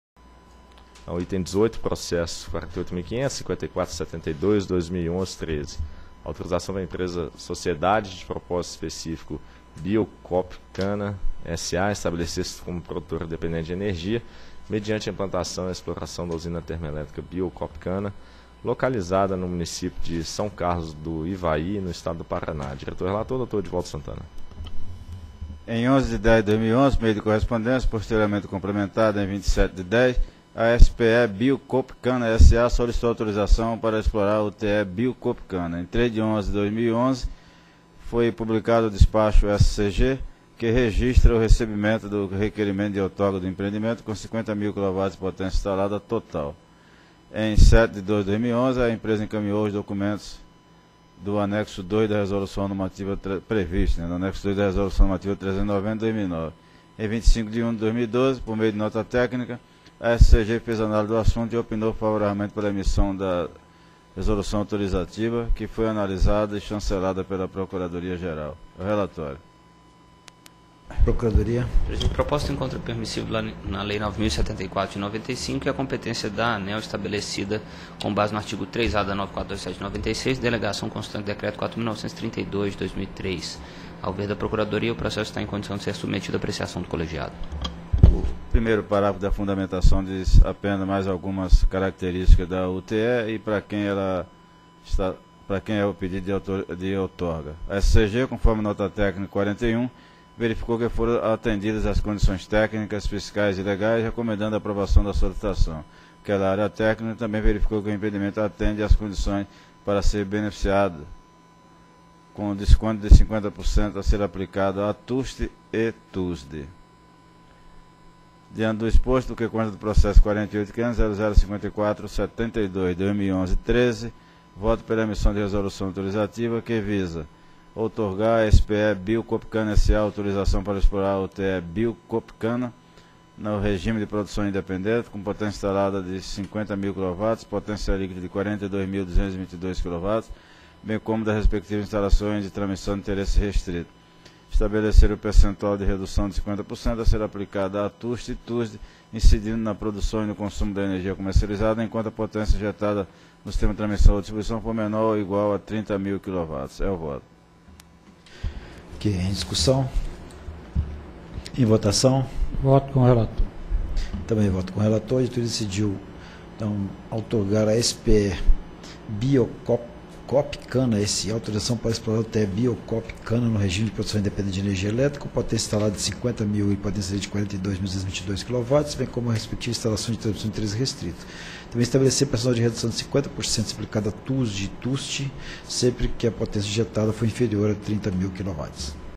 Item 18 - 4ª Reunião Pública Ordinária ANEEL 07/02/2012
Item 18. Processo nº: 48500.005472/2011-13. Assunto: Autorização para a Empresa Sociedade de Propósito Específico -- SPE Bio Coopcana S.A. estabelecer-se como Produtora Independente de Energia Elétrica, mediante a implantação e exploração da Usina Termelétrica -- UTE Bio Coopcana, localizada no município de São Carlos do Ivaí, no estado do Paraná. Área Responsável: Superintendência de Concessões e Autorizações de Geração -- SCG.